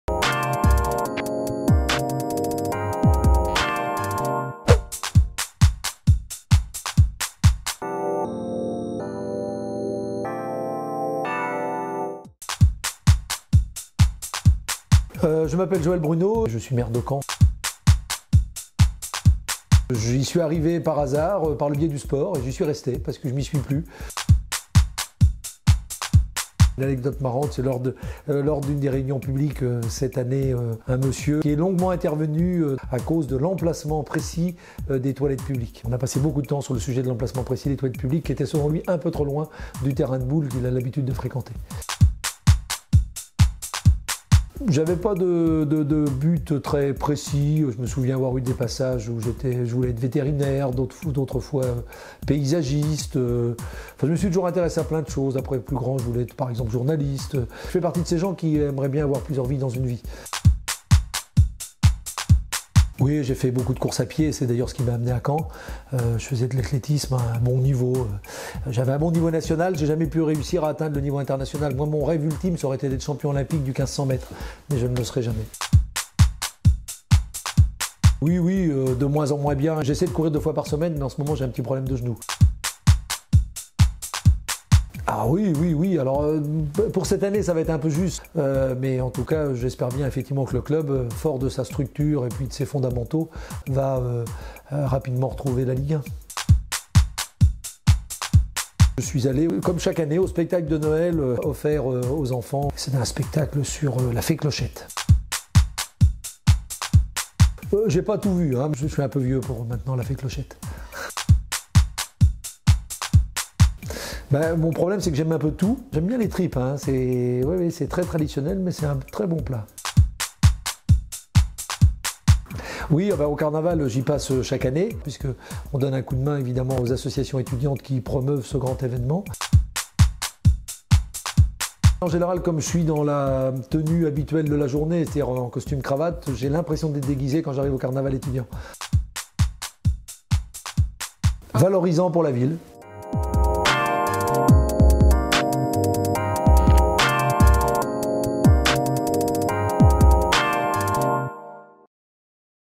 Caen M'aime - Joël Bruneau
Aujourd'hui on vous présente l'interview de Joël Bruneau ! Mais vous commencez à nous connaître... on a décidé de casser les codes 👊

Anecdote, loisirs, sports...  Joël Bruneau, l'actuel Maire de Caen s'est confié à nous sur tout ! 🧚

Merci à Joël d'avoir joué le jeu 🥰

À bientôt,
La team Caen M’aime ❤️

Merci à Paul de @La fabrique Coté terre de nous avoir permis de tourner dans ton merveilleux restaurant 😘